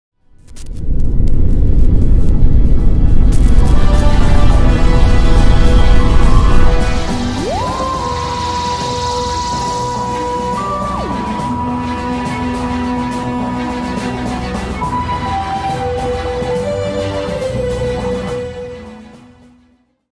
Tardis Animation school assignment
Worked with Cinema 4D and After Effects for a short animation for school.